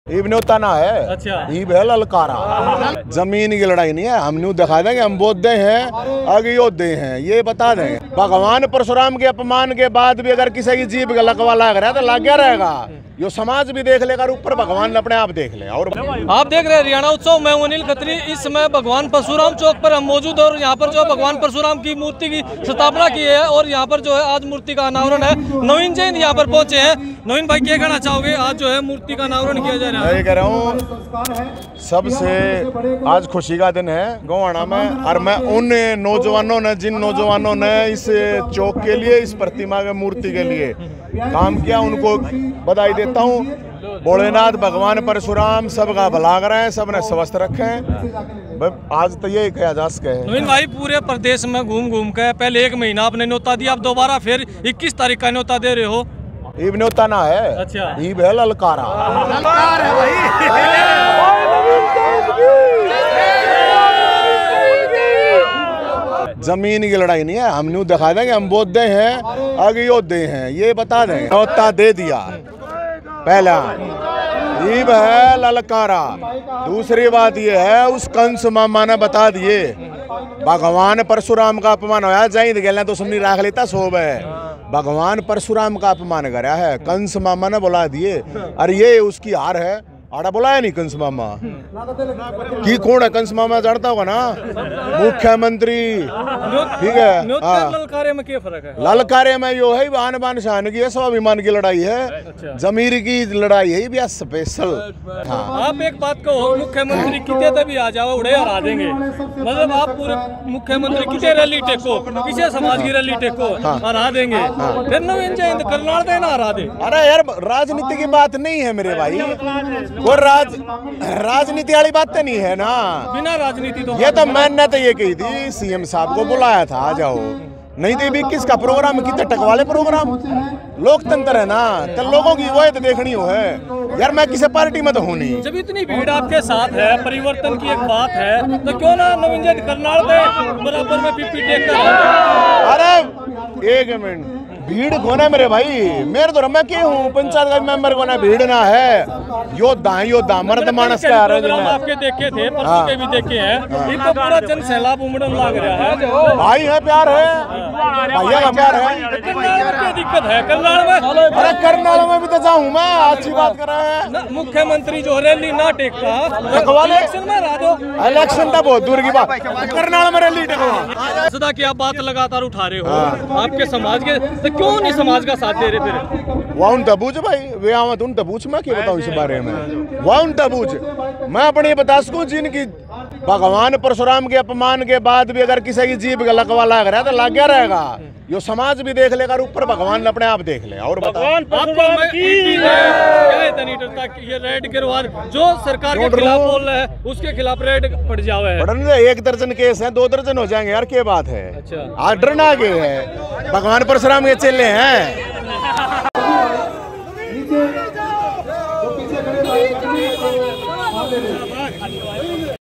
नवीन जयहिंद ने छुड़ाए सीएम खट्टर के छक्के।ब्राह्मण सांसदों को भी सुना गए खरी खरी ।।haryanautsav
नवीन जयहिंद ने छुड़ाए सीएम खट्टर के छक्के।
ब्राह्मण सांसदों को भी सुना गए खरी खरी ।।